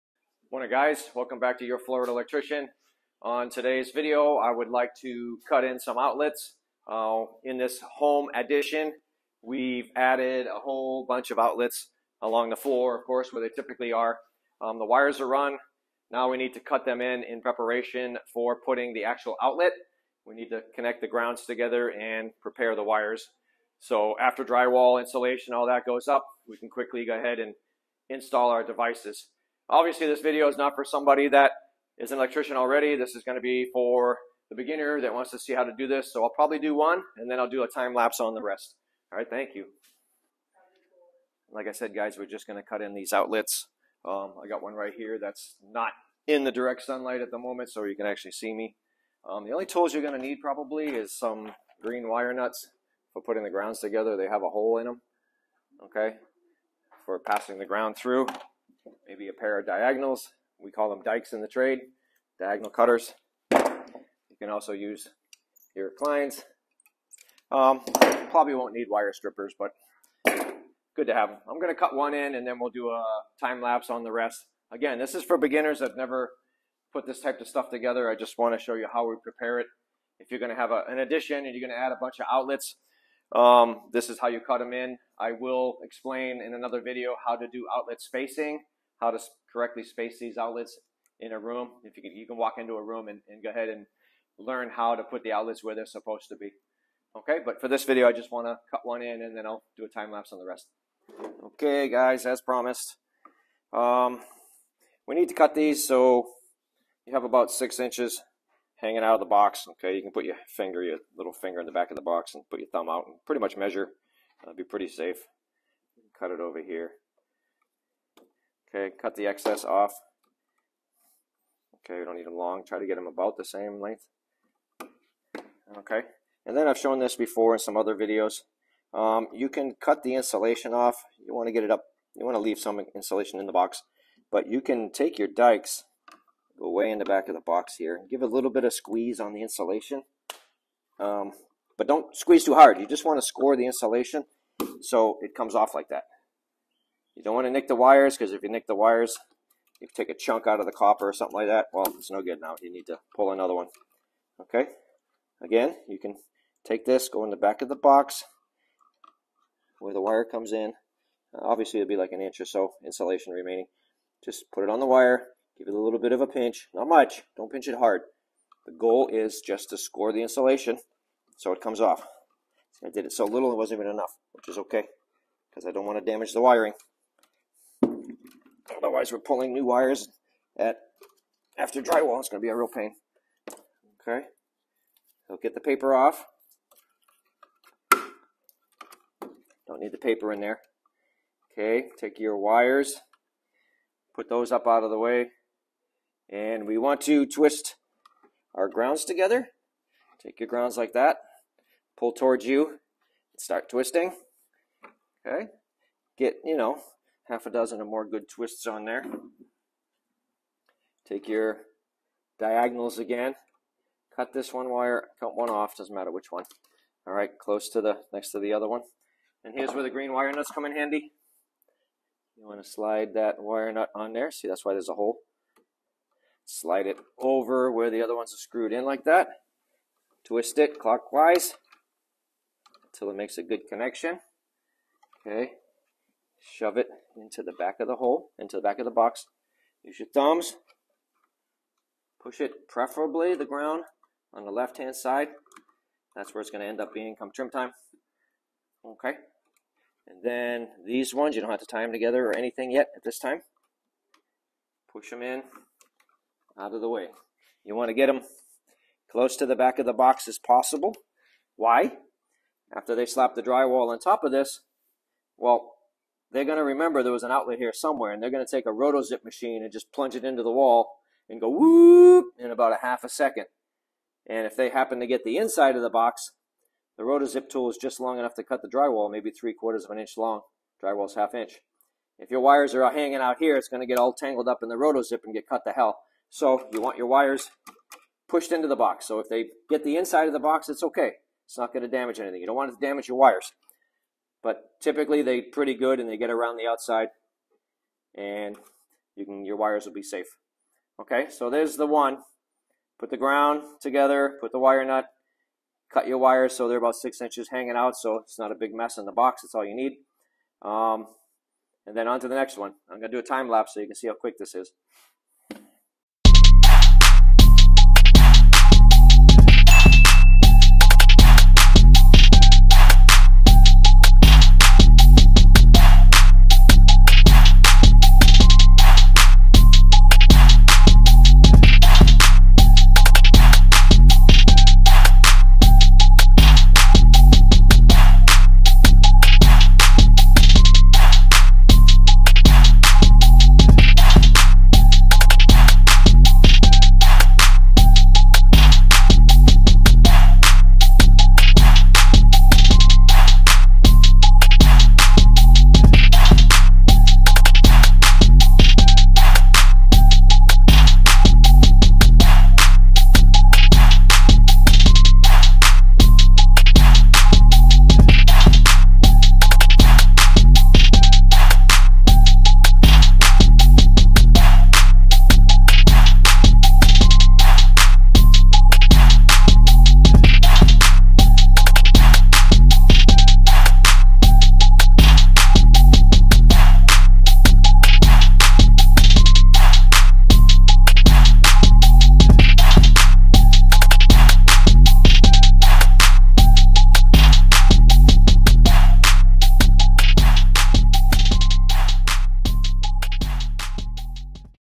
Electrical Rough in - Cutting in for Receptacles
In this video we show you how easy it is to prepare the wiring after electrical rough in to cut in for the receptacles. This video is intended for the beginner to be exposed to how this is done at the rough in stage of home building.

00:00 - Introduction
00:53 - Tools Needed
01:43 - Cutting in
05:17 - Time Lapse